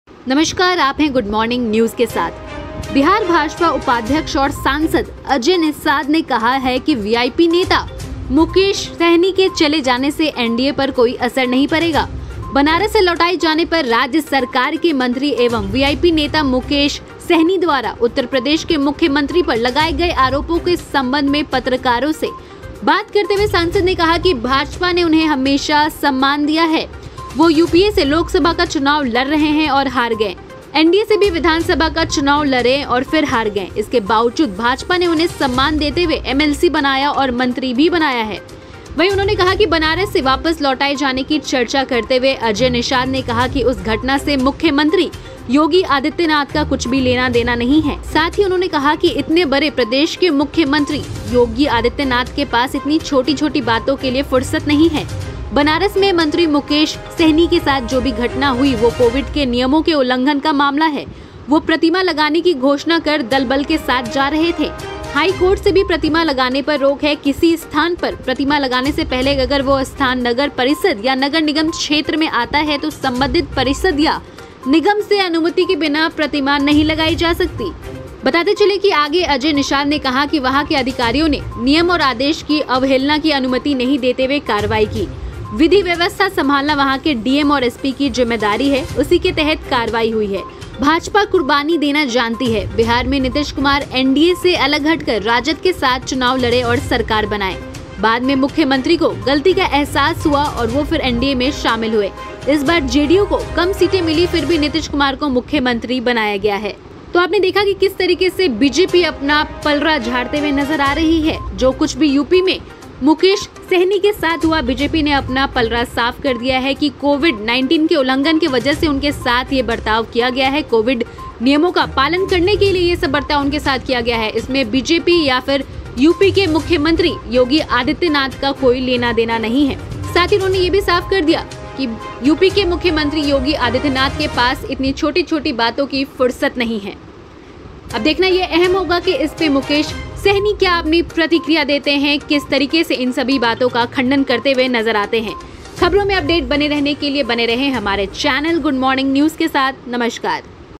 BJP ने मुकेश सहनी को बताया हारा हुआ नेता | Mukesh Shahini
अनुभव रखने वाले जाने-माने जर्नलिस्‍टों की टीम इसको लीड करती हैंबिहार भाजपा उपाध्यक्ष और सांसद अजय निषाद ने कहा है कि वीआईपी नेता मुकेश सहनी के चले जाने से एनडीए पर कोई असर नहीं पड़ेगा। बनारस से लौटाए जाने पर राज्य सरकार के मंत्री एवं वीआईपी नेता मुकेश सहनी द्वारा उत्तर प्रदेश के मुख्यमंत्री पर लगाए गए आरोपों के संबंध में पत्रकारों से बात करते हुए सांसद ने कहा है कि भाजपा ने उन्हें हमेशा सम्मान दिया है। वे यूपीए से लोकसभा का चुनाव लड़े और हार गए। एनडीए से भी विधानसभा का चुनाव लड़े और फिर हार गए। इसके बावजूद भाजपा ने उन्हें सम्मान देते हुए एमएलसी बनाया और मंत्री भी बनाया।बनारस से वापस लौटाए जाने की चर्चा करते हुए अजय निषाद ने कहा कि उस घटना से मुख्यमंत्री योगी आदित्यनाथ का कुछ भी लेना-देना नहीं है।  इतने बड़े प्रदेश के मुख्यमंत्री योगी आदित्यनाथ के पास इतनी छोटी-छोटी बातों के लिए फुर्सत नहीं है। बनारस में मंत्री मुकेश सहनी के जो भी घटना हुई, वह कोविड के नियमों के उल्लंघन का मामला है। वे प्रतिमा लगाने की घोषणा कर दल-बल के साथ जा रहे थे। हाई कोर्ट से भी प्रतिमा लगाने पर रोक है। किसी स्थान पर प्रतिमा लगाने से पहले अगर वह स्थान नगर परिषद या नगर निगम क्षेत्र में आता है तो संबंधित परिषद या निगम से अनुमति के बिना प्रतिमा नहीं लगायी जा सकती है।

वहां के अधिकारियों ने नियम और आदेश की अवहेलना की अनुमति नहीं देते हुए कार्रवाई की। विधि व्यवस्था संभालना वहां के डीएम और एसपी की जिम्मेदारी है। उसी के तहत कार्रवाई हुई है।  भाजपा कुर्बानी देना जानती है। बिहार में नीतीश कुमार एनडीए से अलग हटकर राजद के साथ चुनाव लड़े और सरकार बनाए। बाद में मुख्यमंत्री को गलती का अहसास हुआ और वे फिर एनडीए में शामिल हुए। इस बार जेडीयू को कम सीटें मिलीं, फिर भी नीतीश कुमार को मुख्यमंत्री बनाया गया।